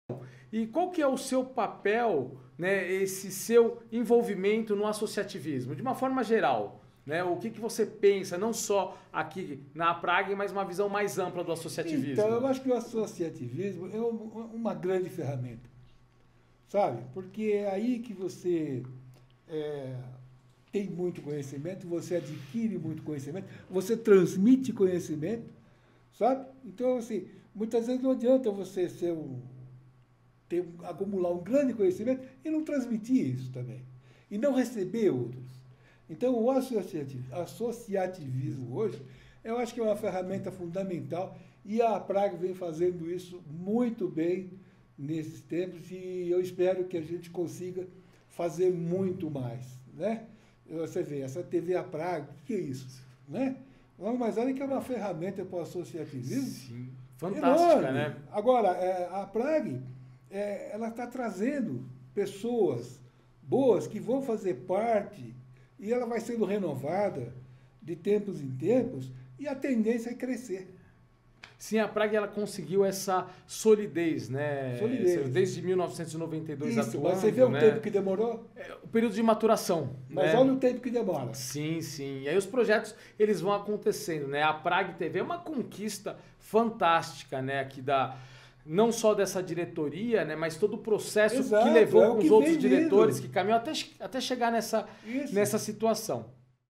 ASSOCIATIVISMO É UMA GRANDE FERRAMENTA!
Cortes PodCast De papo com PCO #8
APRAG - Associação dos Controladores de Vetores e Pragas Urbanas